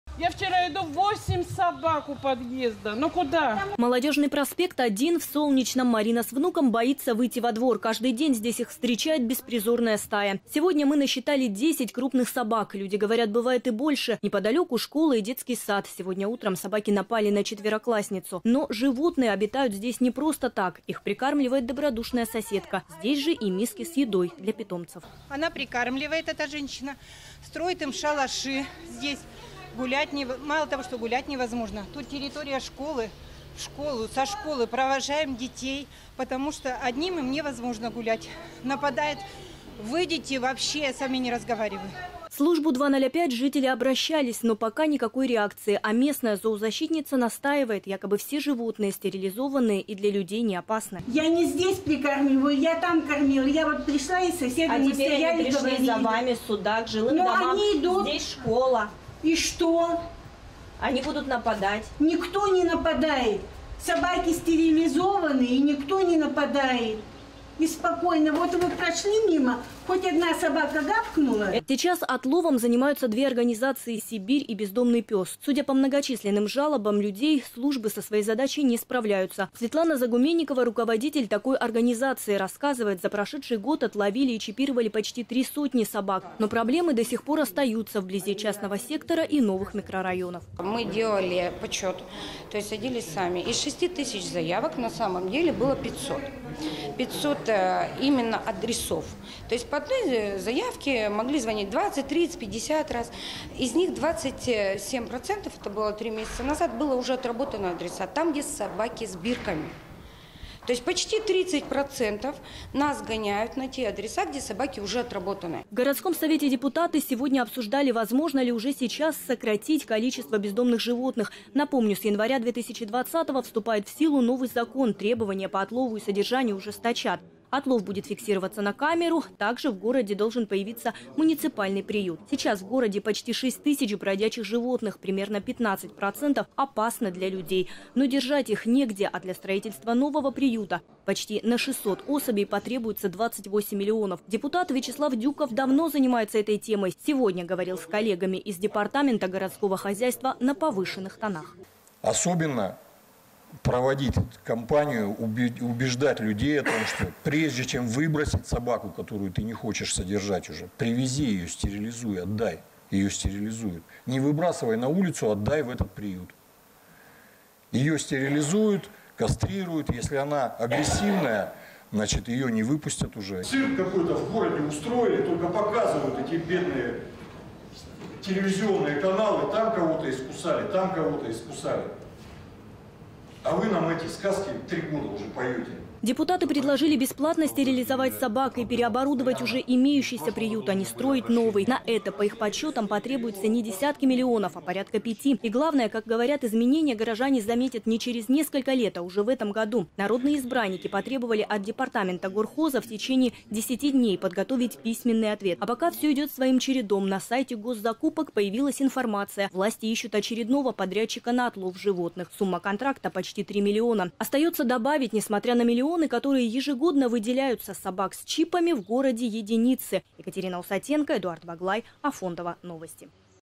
Стая бездомных собак напала на девочку в Солнечном в районе 115-ой школы
По словам жителей, этот случай далеко не первый.

Во дворе дома на проспекте Молодежном, 1, бездомные животные обитают постоянно. Здесь их прикармливает сердобольная жительница.

Ежедневно во дворе насчитывают около десятка собак. Соседи возмущены: ночами они слушают лай, а по утрам боятся отпускать детей одних в школу и водить малышей в детский сад.

Жители говорят, в службу «005» обращались, но пока никакой реакции. А местная зоозащитница настаивает на своем: якобы, все животные стерилизованные и для людей не опасны.

С подробностями Екатерина Усатенко.